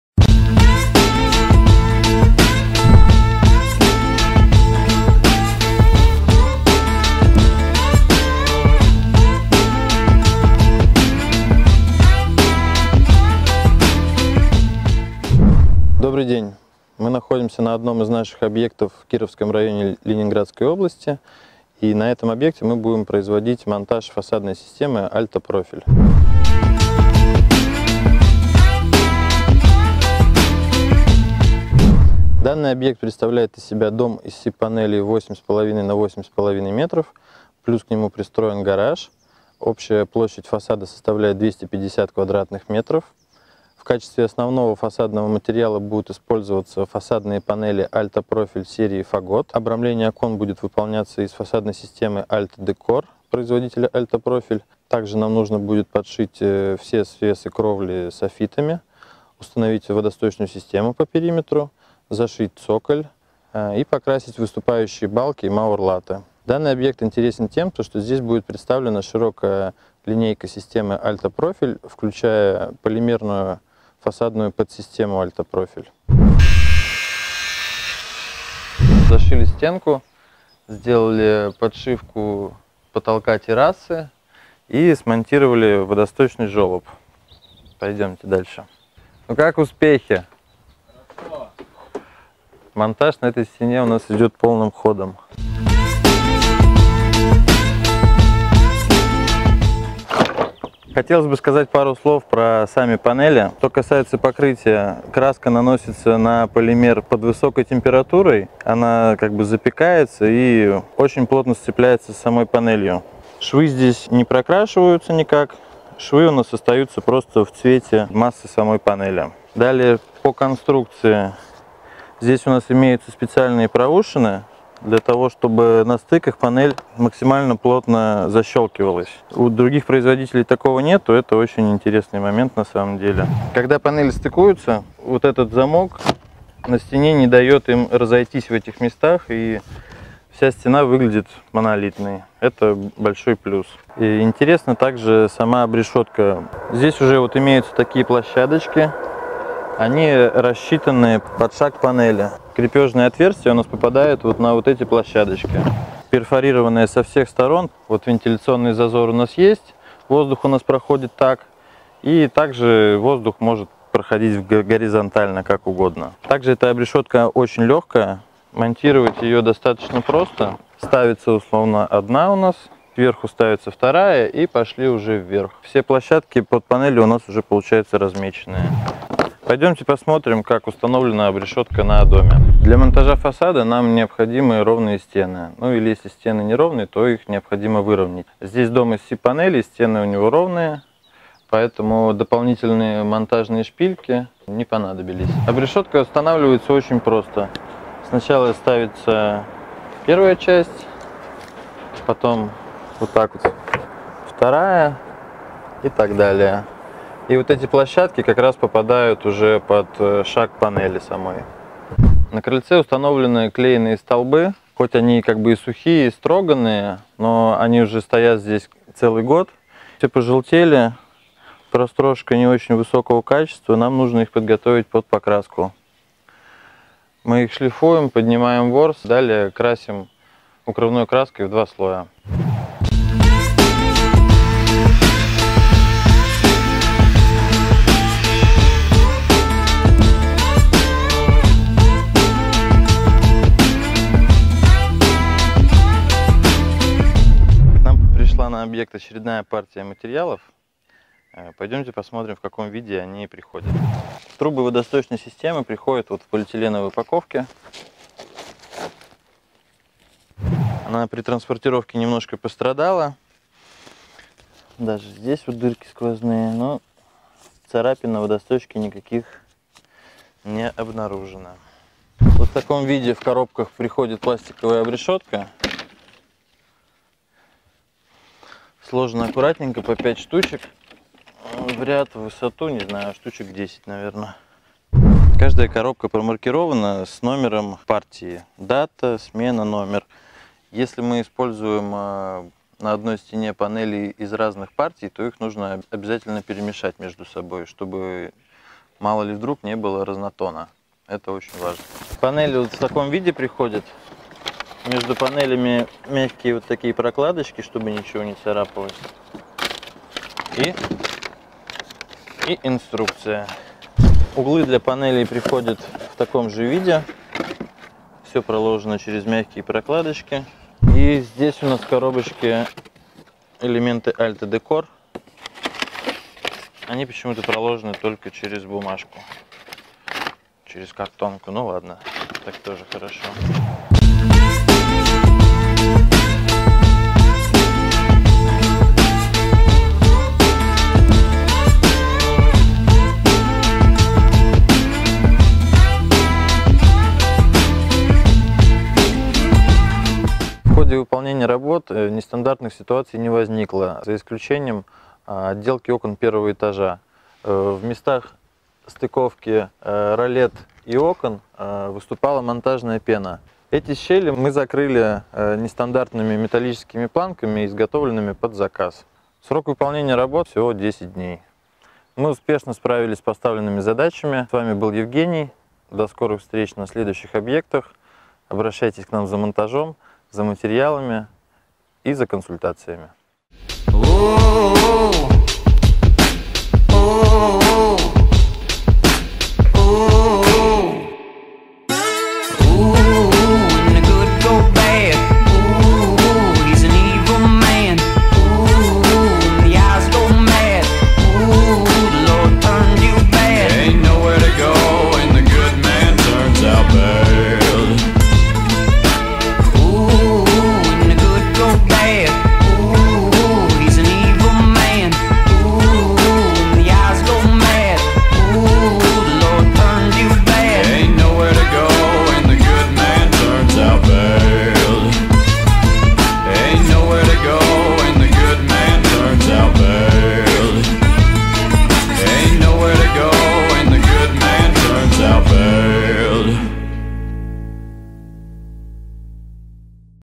Отделка дома фасадными панелями Альта-Профиль
В этом ролике вы увидите монтаж фасадных панелей с использованием специальной системы крепления фасадов компании Альта - Профиль.